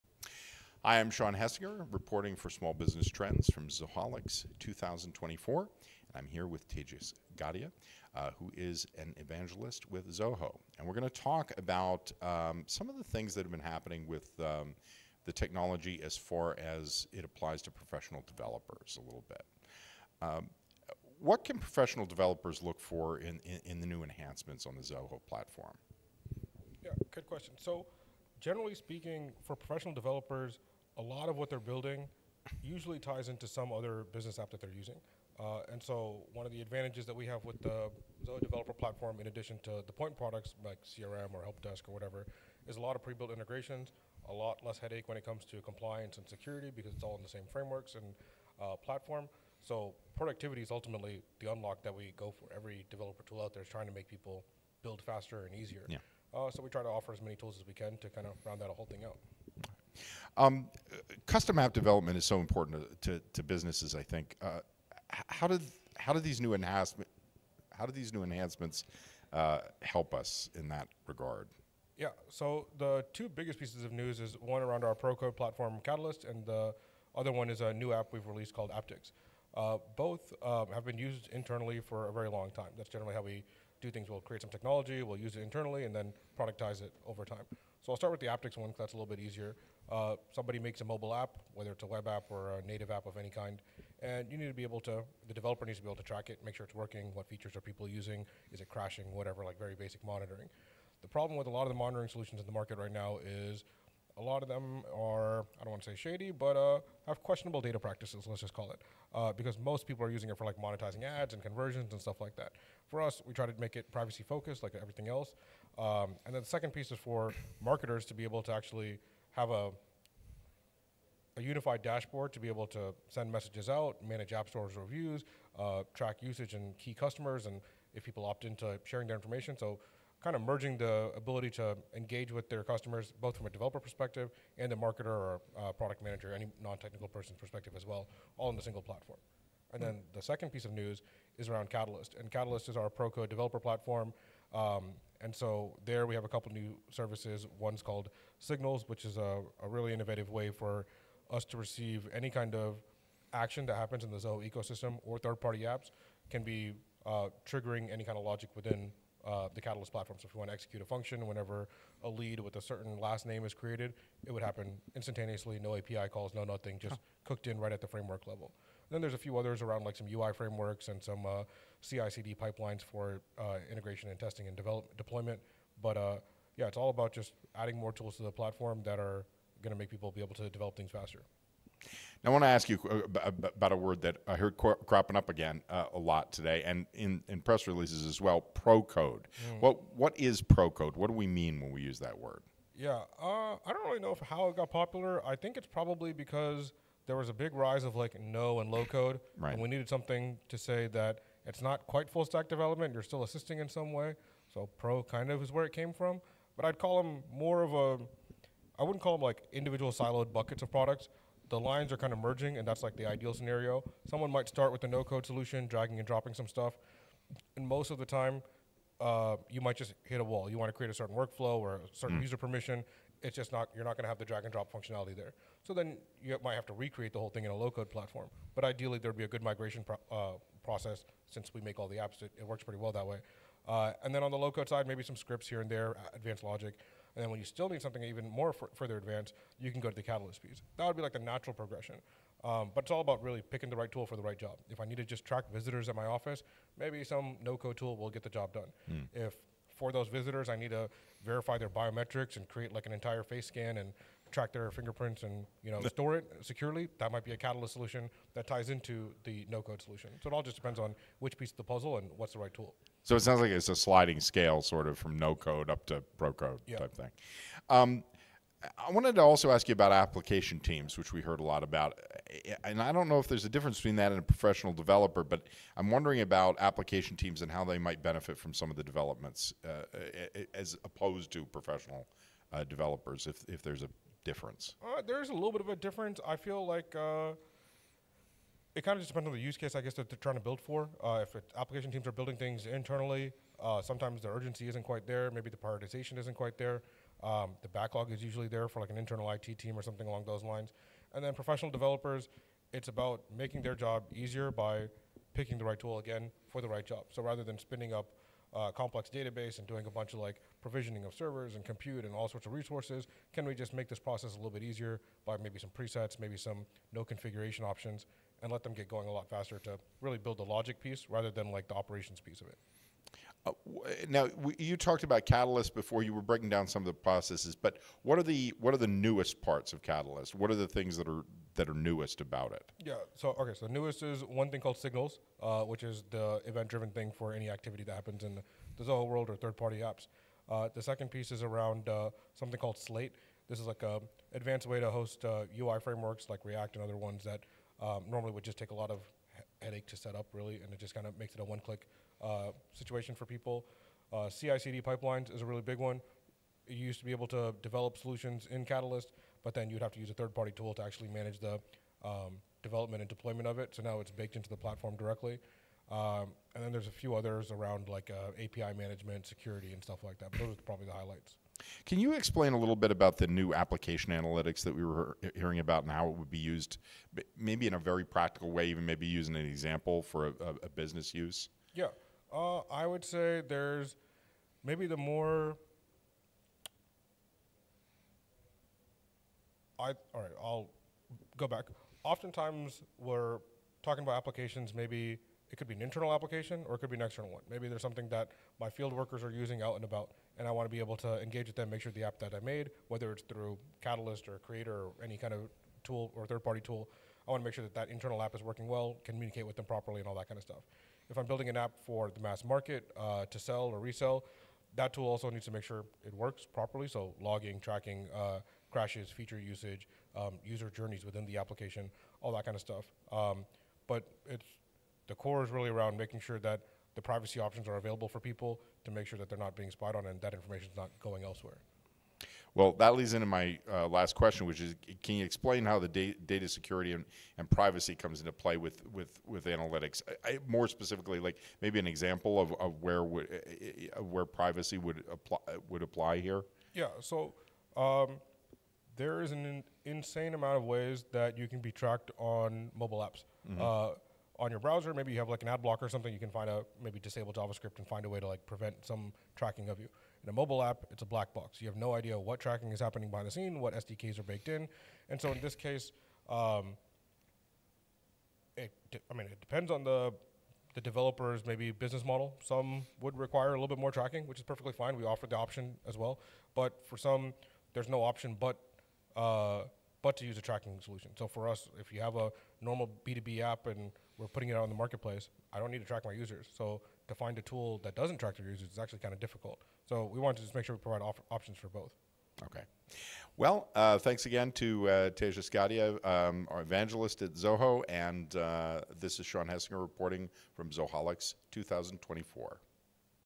Tejas Gadhia on Zoho’s Pro-Code Enhancements and New Developer Tools at Zoholics24
Join us for an enlightening interview with Tejas Gadhia from Zoho at Zoholics 2024, where we delve into the exciting new enhancements on Zoho’s platform that cater to professional developers. Tejas explains the concept of pro-code and how it empowers developers to create custom applications that are critical for modern businesses. We also discuss the benefits for application teams and take a closer look at Zoho’s latest offerings on Catalyst, including new application analytics and their significance in business. This interview is a must-watch for developers and businesses looking to leverage the latest in technology and application development.